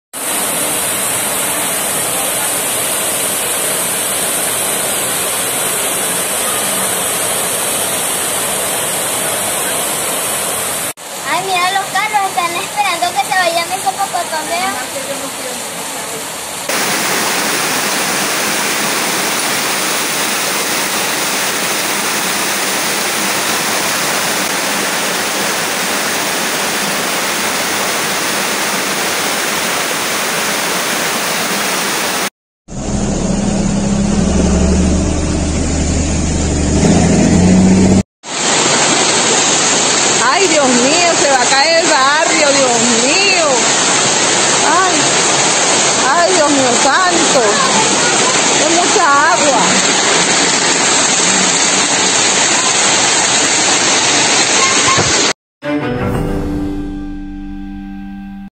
Lluvias inundan calles de Caracas por segundo día consecutivo
Este 11 de agosto se registraron fuertes lluvias en la Gran Caracas, que, por segundo día consecutivo dejaron varias callesinundadas.

Según reporte de usuarios en Twitter, sectores como LaVega, El Valle y San Martín (municipio Libertador), el kilómetro cero de la carretera Panamericana vía Los Teques (estado Miranda) y el barrio Puma Rosa, ubicado en la carretera Caracas – La Guaira, fueron algunos de los más afectados por las lluvias que azotaron en horas de la tarde.

Imágenes de sol651, Lennys Vargas

Contribuye a nuestro medio sumándote a nuestra membresía como SuperAliado/a o dando un aporte único.Visita ¡Hazte SuperAliado!